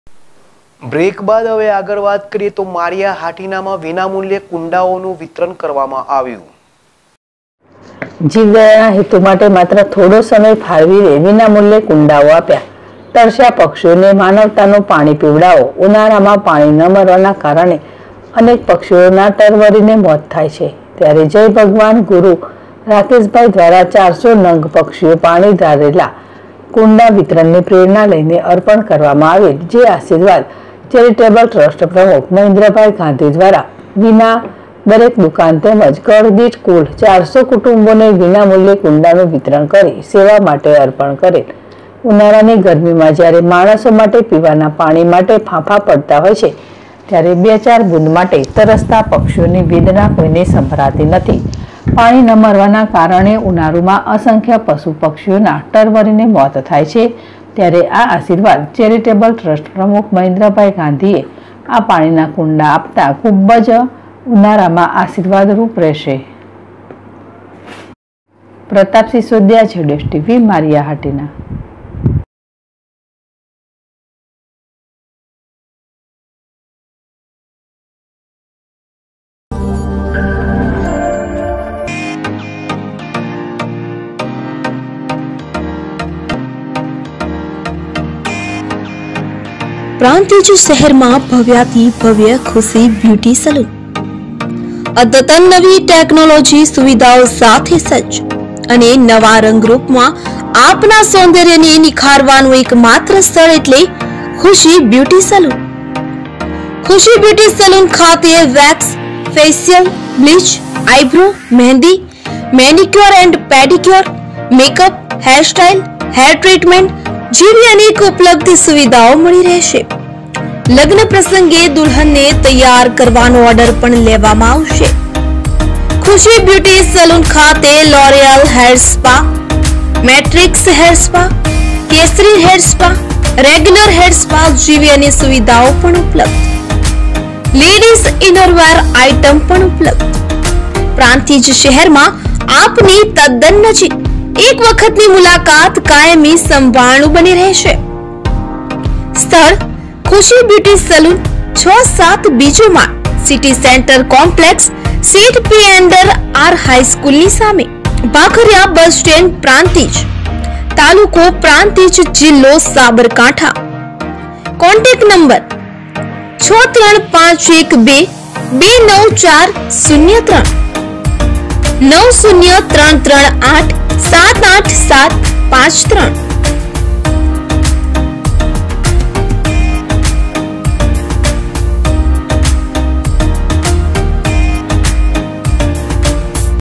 માળીયા હાટીના માં વિનામૂલ્યે કુંડાનું વિતરણજીવદયાના હેતુ માટે માત્ર થોડો સમય ફાળવીને વિના મૂલ્યે
માળીયા હાટીના માં વિનામૂલ્યે કુંડાનું વિતરણ

જીવદયાના હેતુ માટે માત્ર થોડો સમય ફાળવીને વિના મૂલ્યે કુંડાઓ આપ્યા